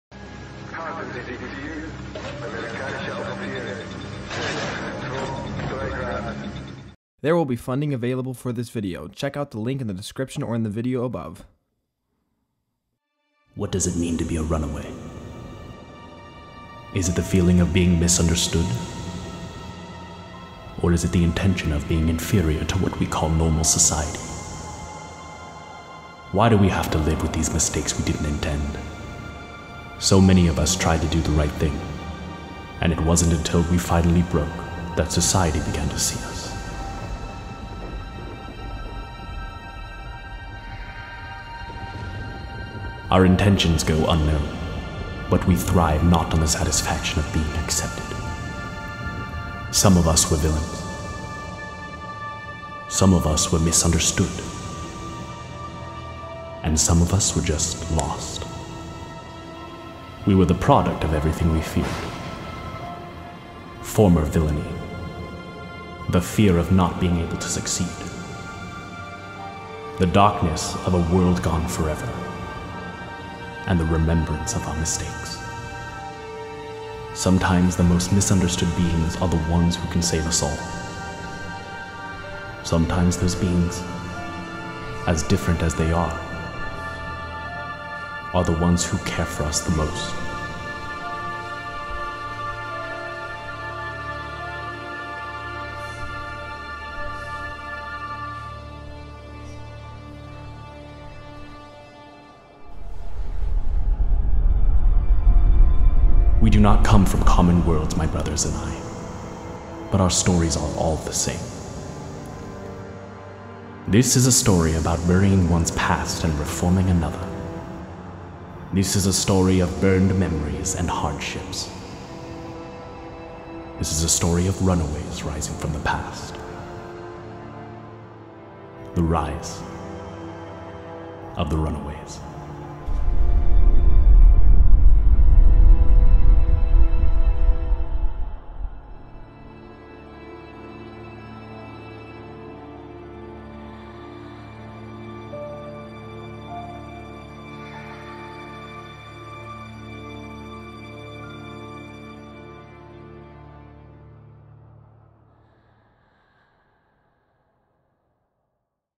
Rise of the Runaways- The beginning chapter promo
This is another promotion of my series, "Rise of the Runaways" that ties in some of the emotional reasons as to why these characters are together. When the series starts, it'll make perfect sense!

Share this around wherever you can! This series needs as much support as it can get!